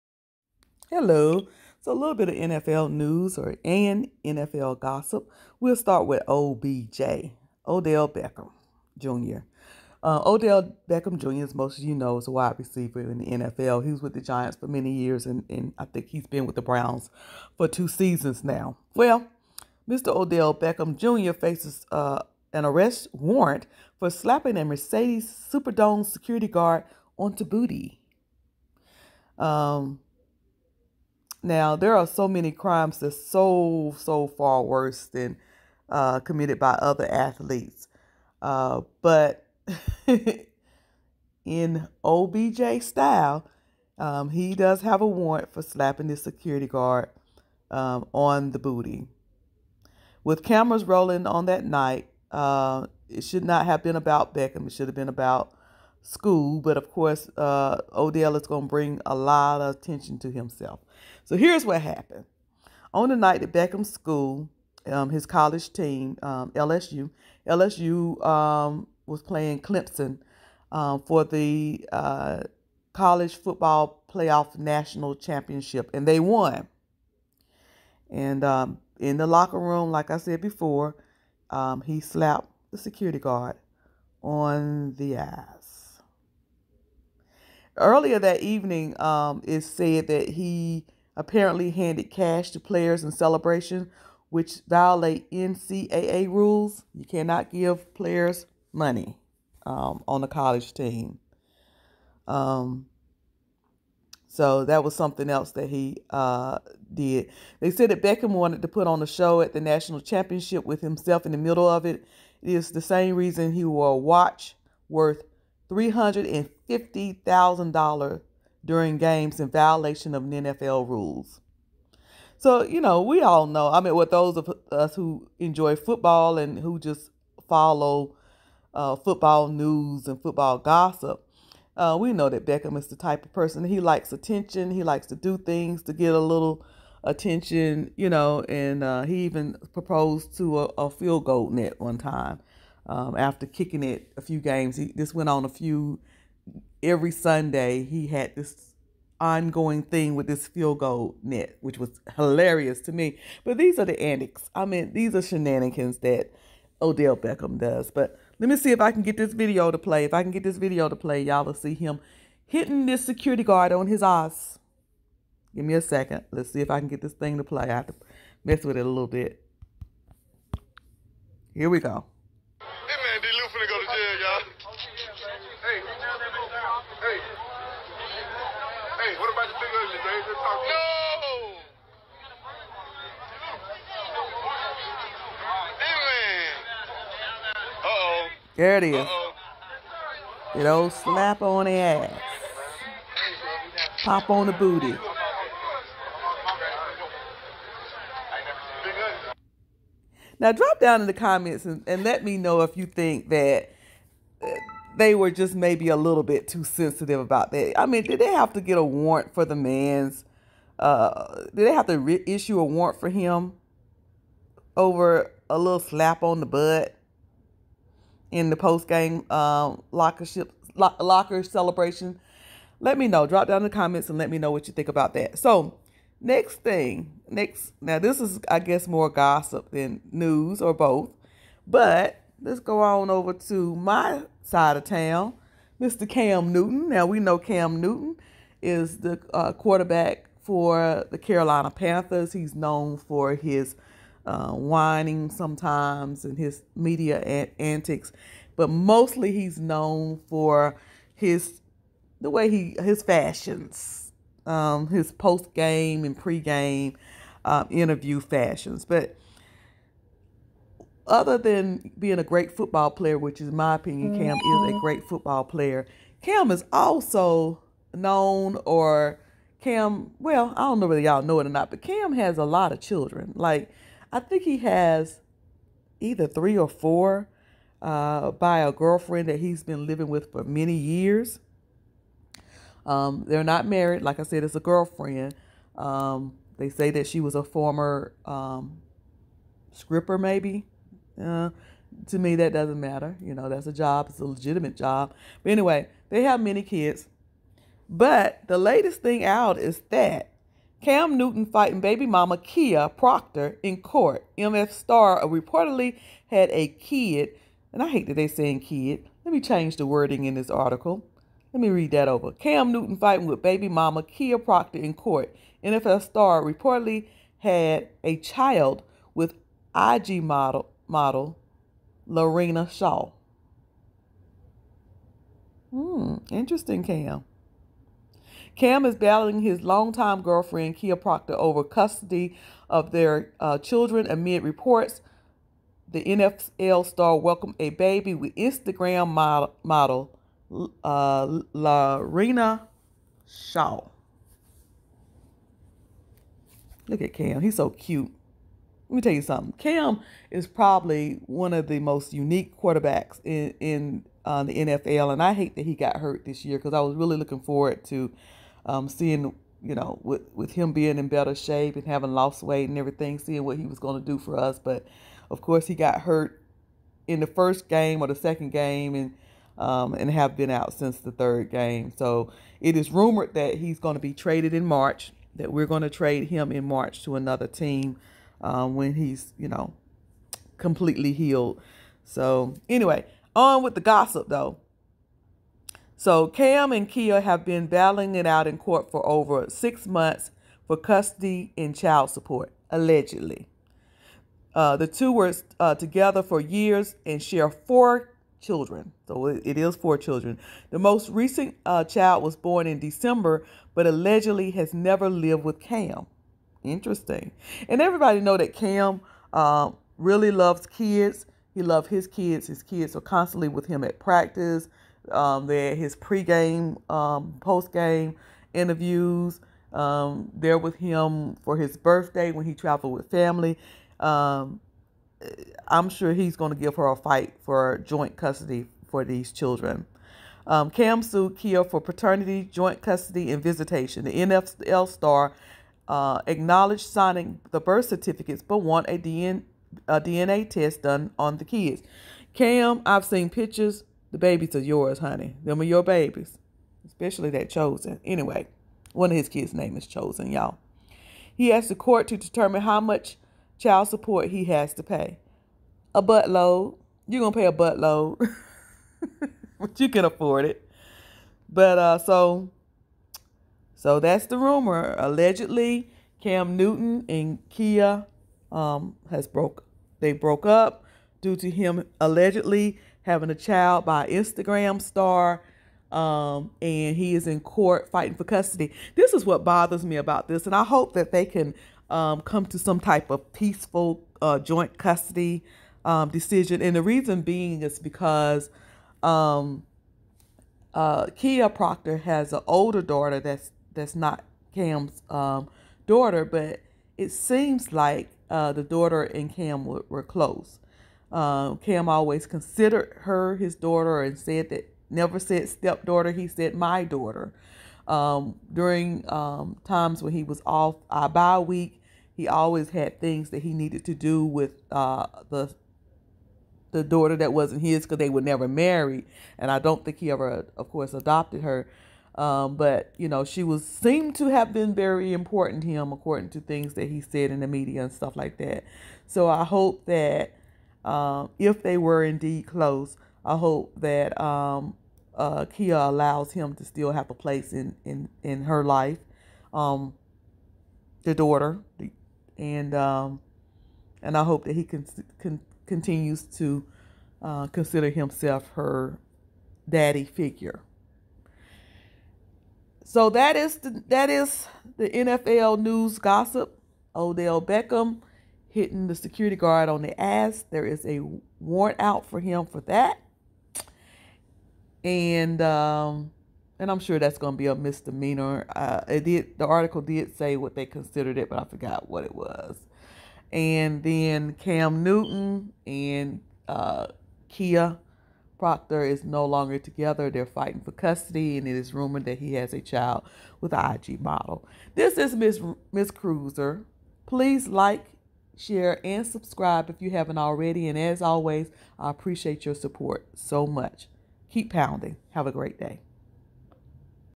Odell Beckham Jr. & Cam Newton NFL news & Gossip
Cops Issue Arrest Warrant for Browns’ Odell Beckham Jr.  slapping the butt of a security guard was which caught on video
Cam Newton Allegedly Splits From Girlfriend Kia Proctor After Having Baby With Instagram Model La Reina Shaw